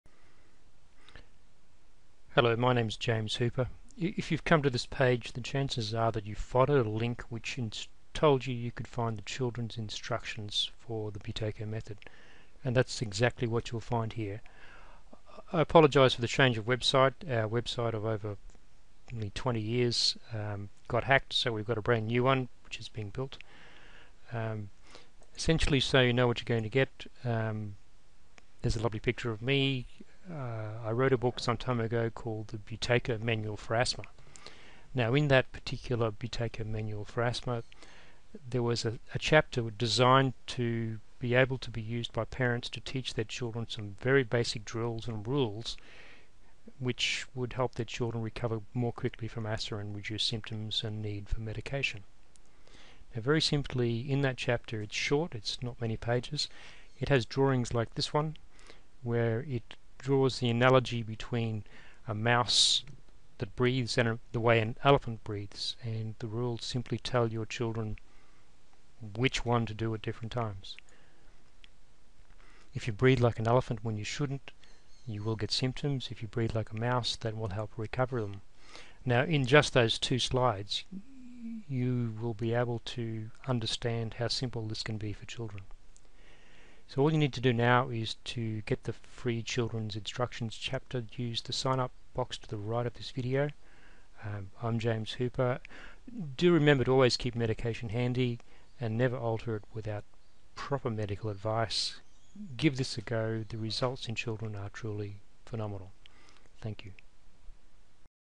Buteyko Method FREE Chldrens Asthma Instructions Download
Visit nqnet.com to get your copy of the free Children's Chapter from James Hooper's Buteyko Manual for Asthma.

It contains all the basic information you need, plus shares exactly what you need to teach your children to almost immediately begin benefitting from this proven way to reduce asthma symptoms and medications.